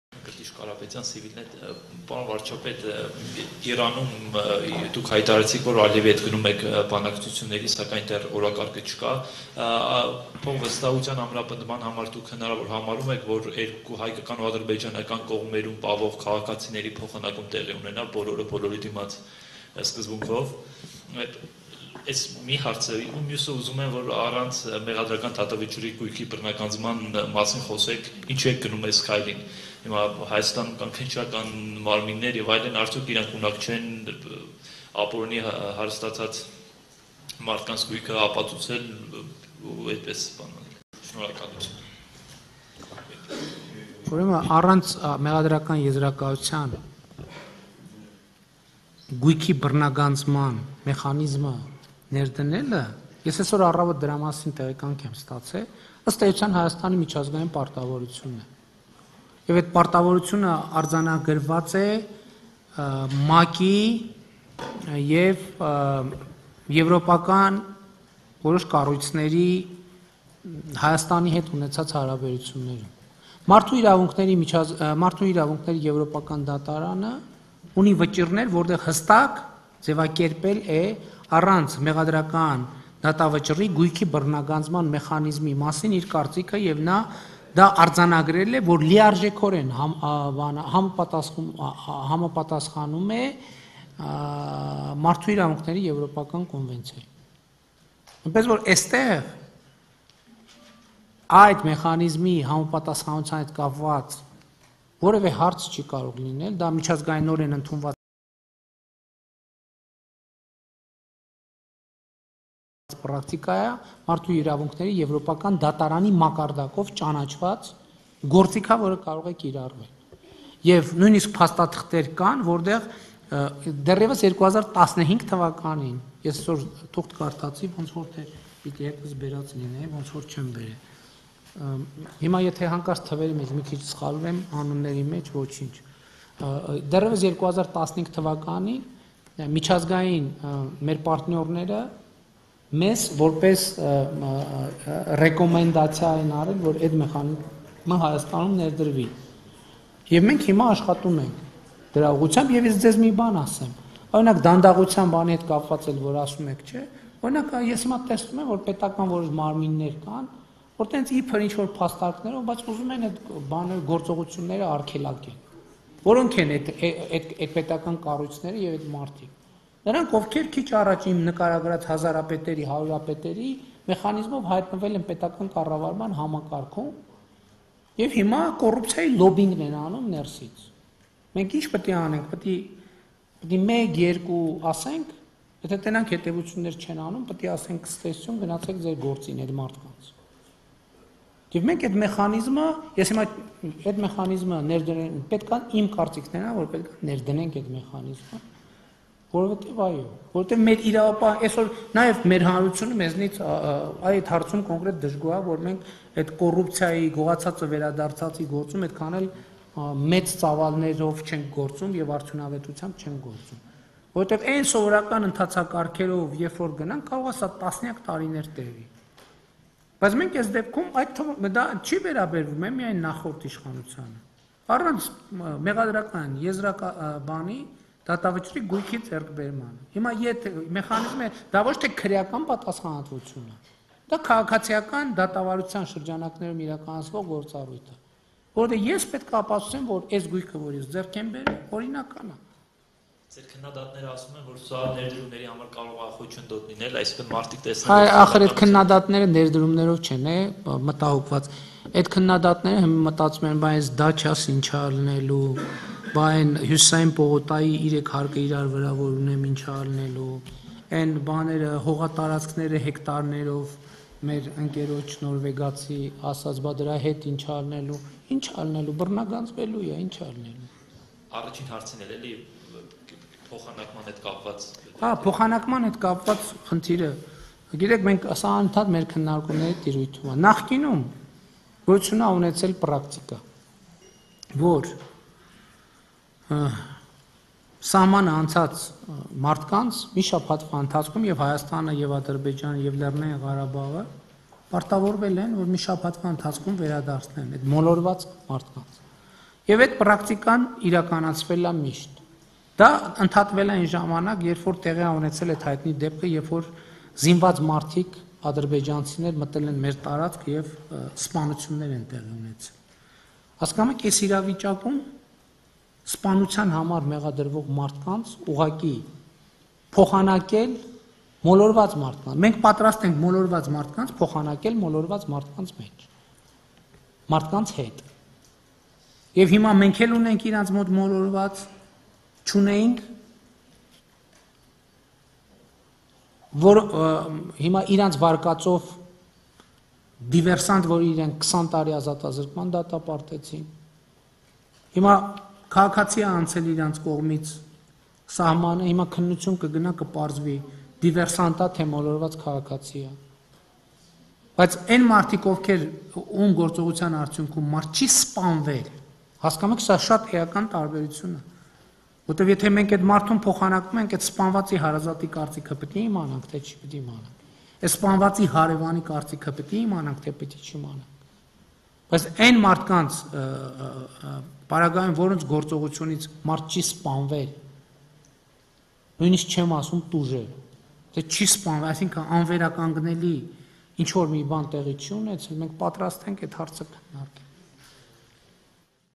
Պատրաստ ենք մոլորված մարդկանց փոխանակել մոլորվածների հետ. վարչապետ
Պատրաստ ենք մոլորված մարդկանց փոխանակել մոլորվածների հետ. վարչապետը՝ պատանդների եւ հանցագործների մասին